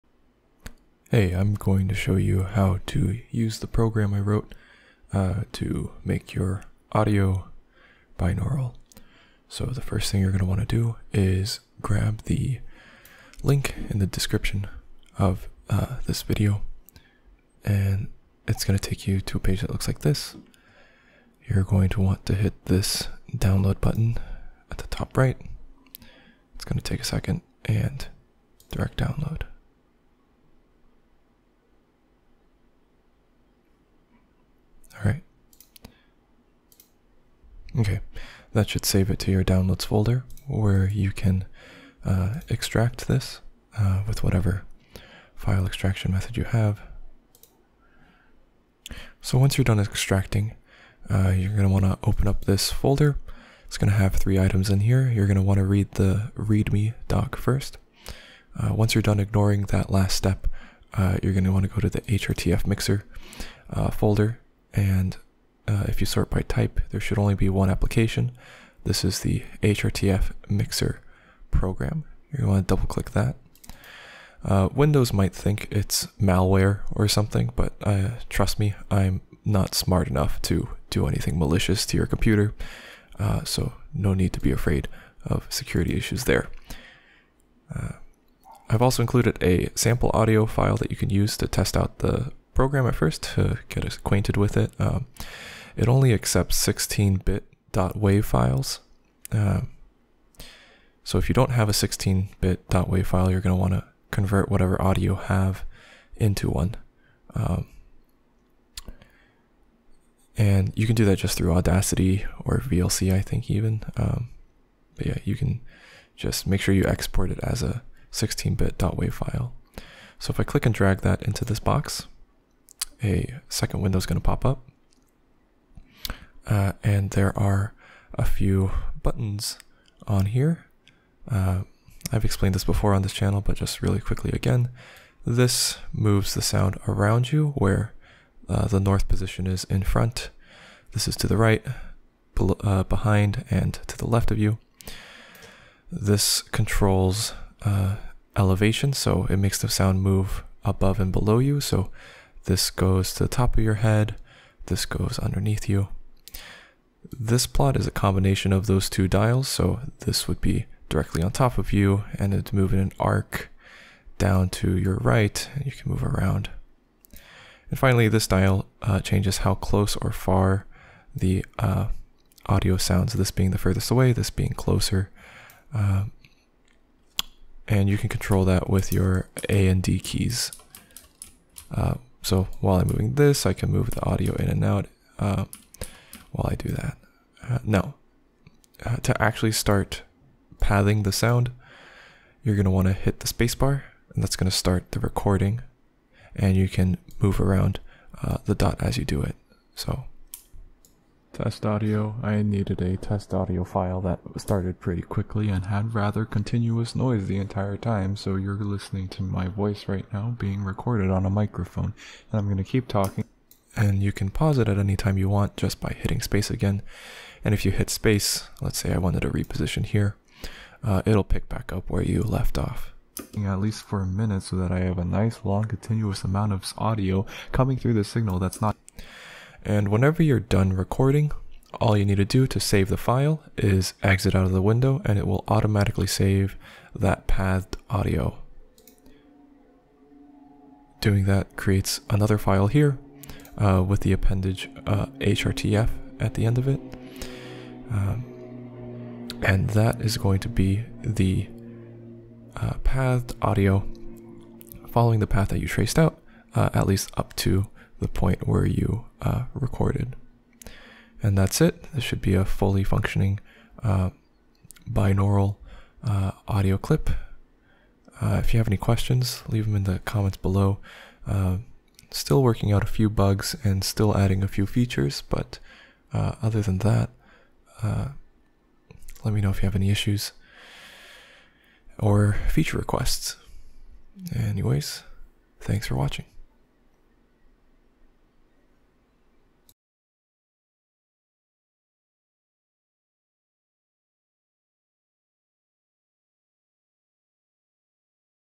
How to Create Binaural Audio Without a Binaural Microphone
I think it should finally be usable at least now. v0.0 and v0.1 had bugs that made it unusable. Thanks!